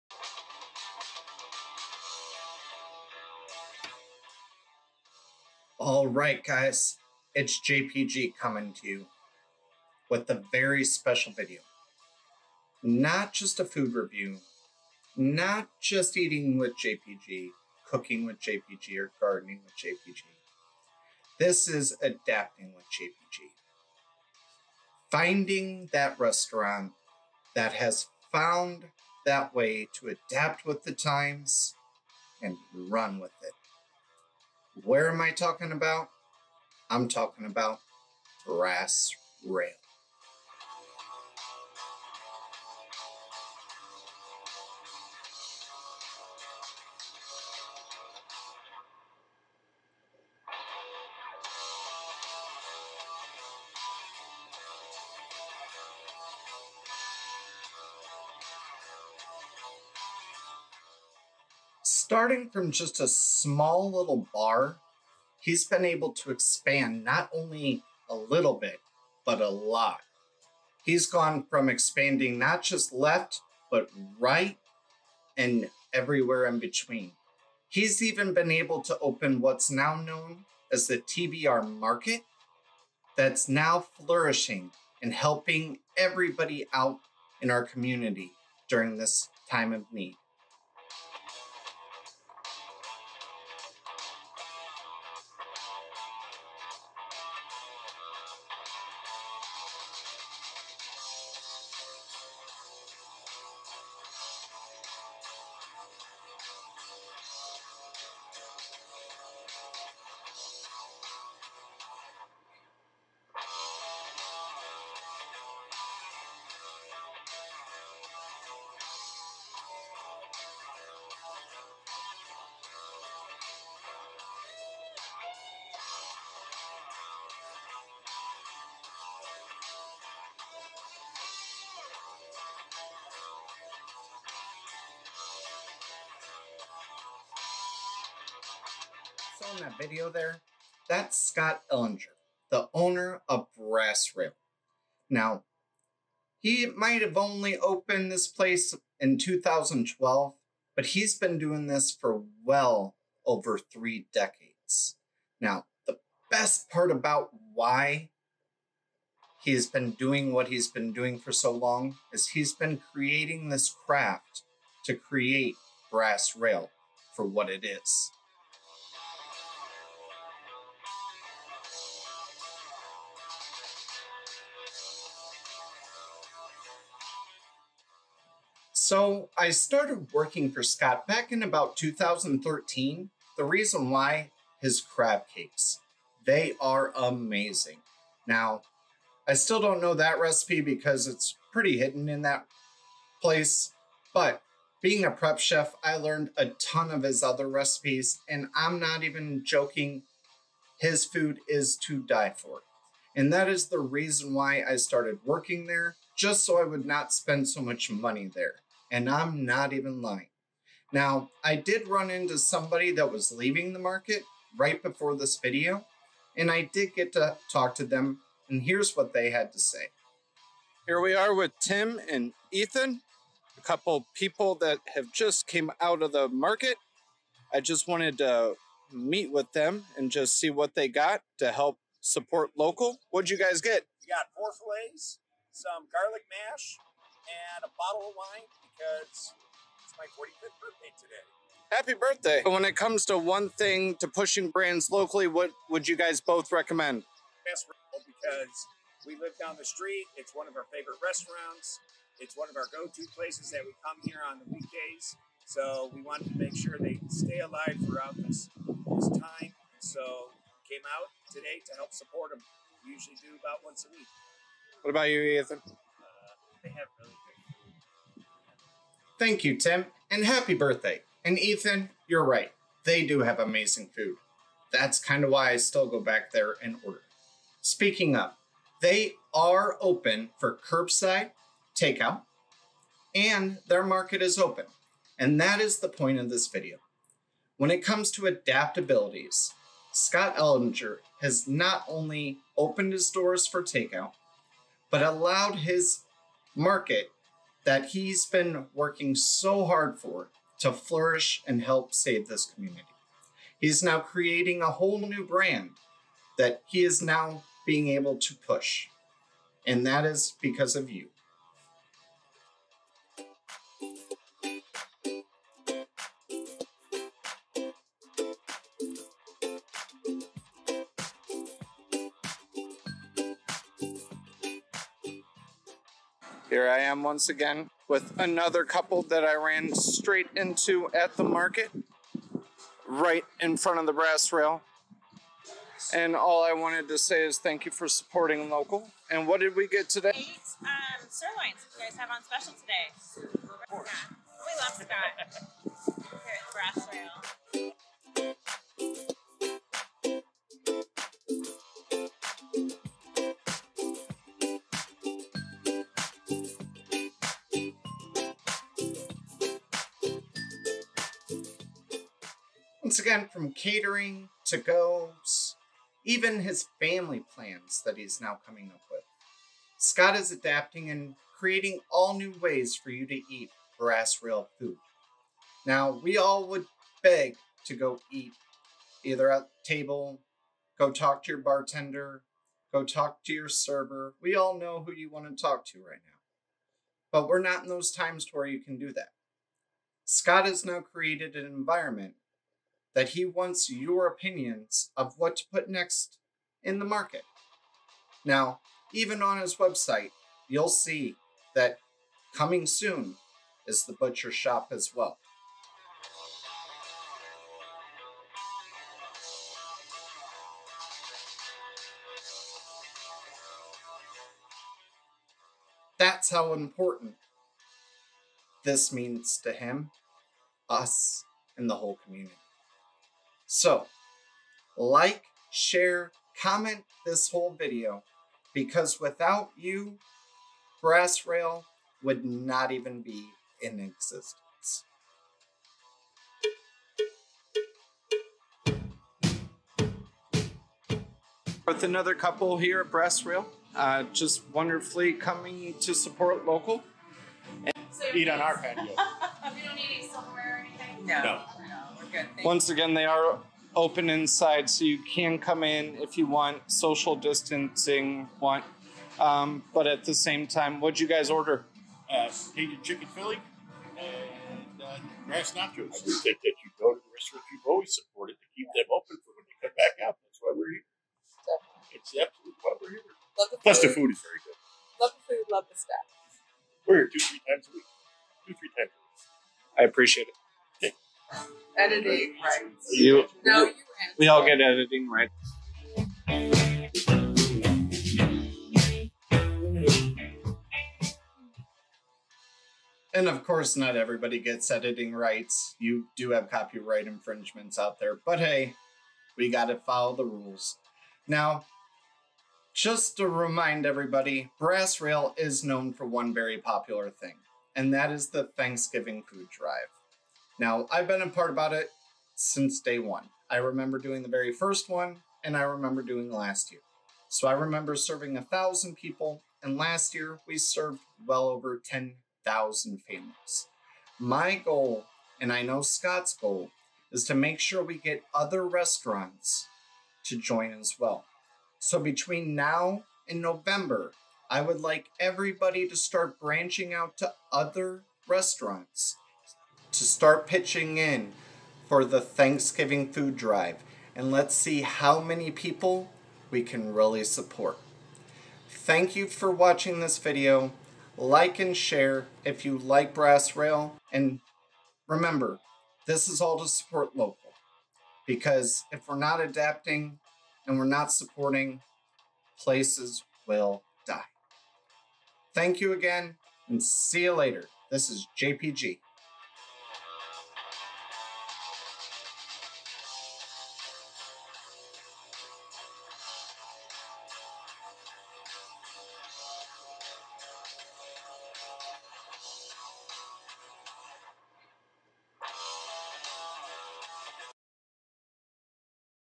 Brass Rail Steakhouse Gives Back To The Community Among-st Current Conditions. Adaption is key.
Brass Rail Steakhouse Gives Back To The Community Among-st Current Conditions. Adaption is key. AdaptingwJpg Jpg_Dcs takes you through the fact that, during these hard times, SupportingLocal, being one community, and truly coming together to becoming a family is so important.